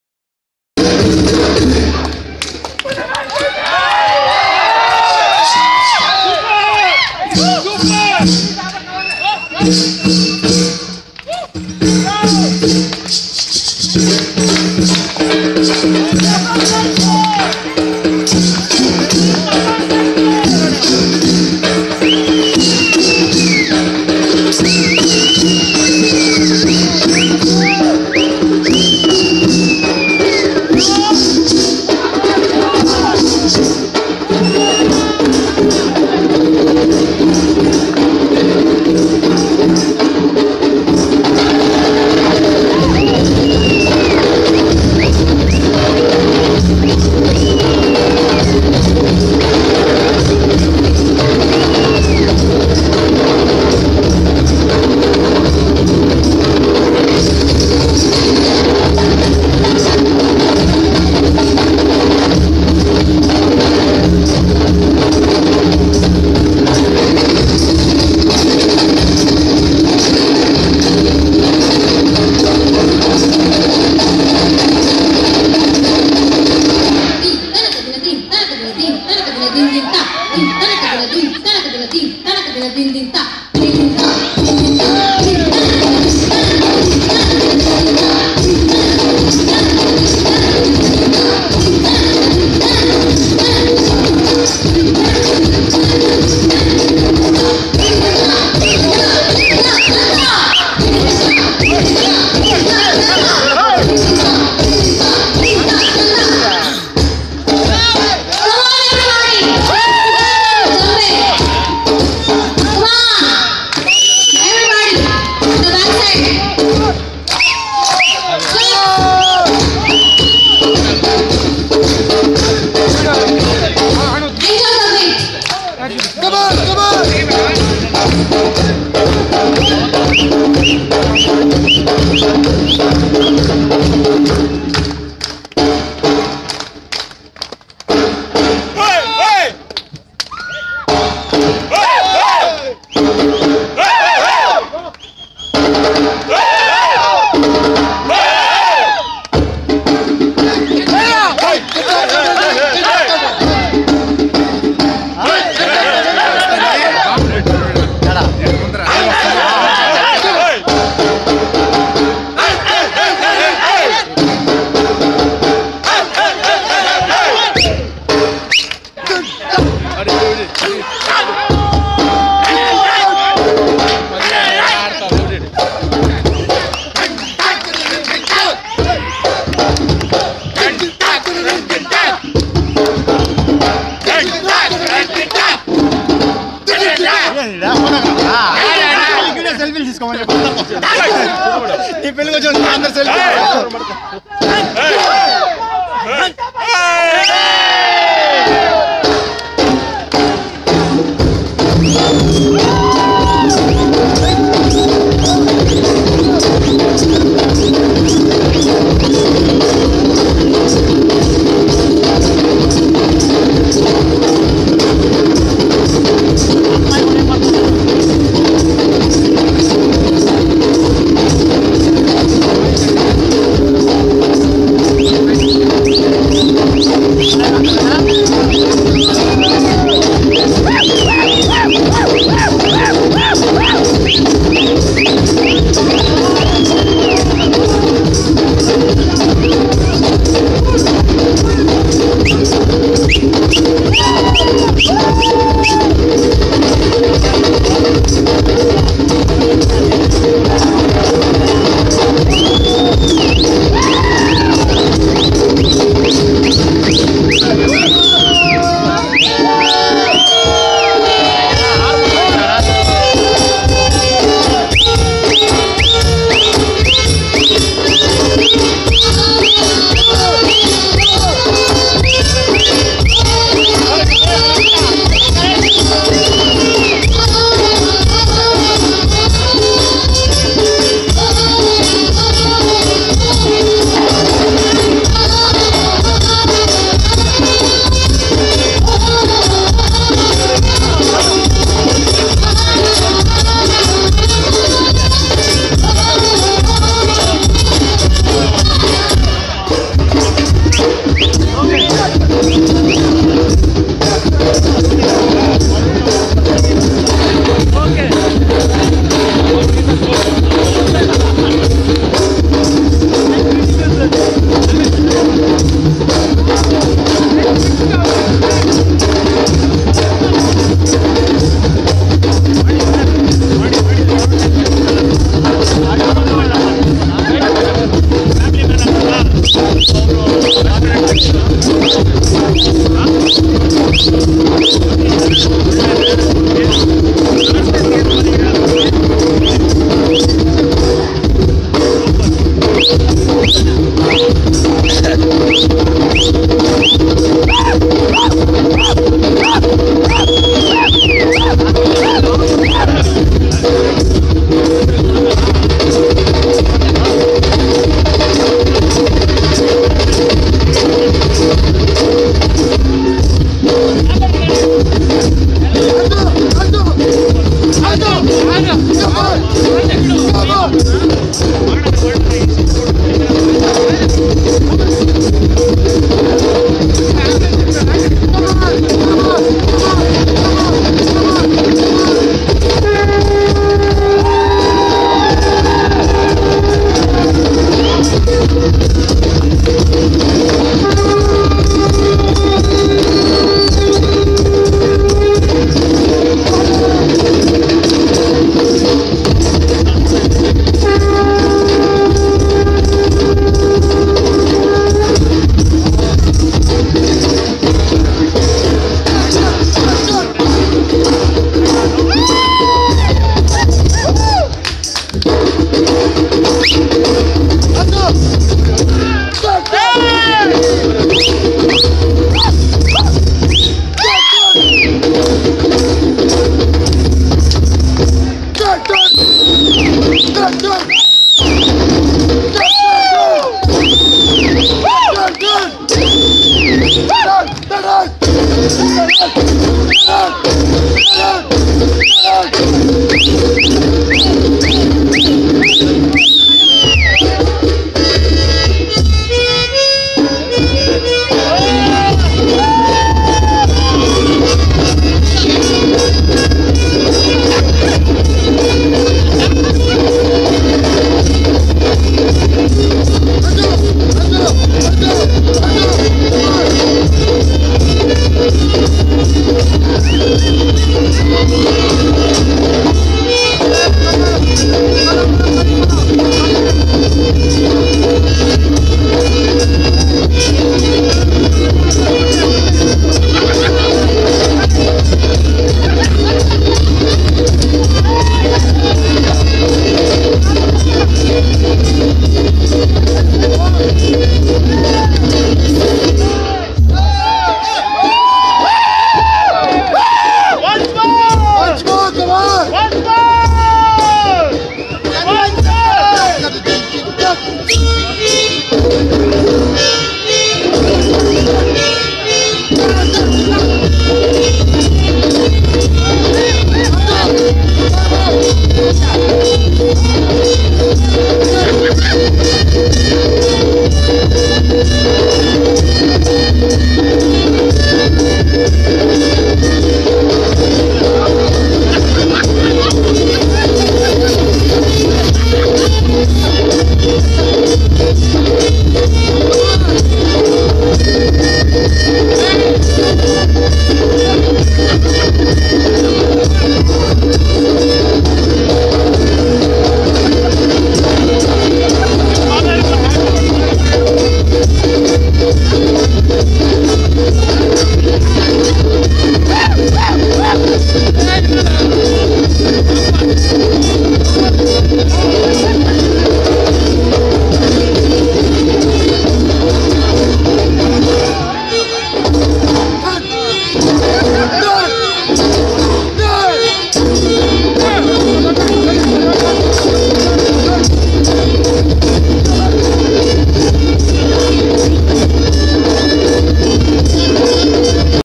Jambe- jalak
Indian folk band welcoming a new year 2018 through folk spirit,  at "The venue"  near Kanakapura.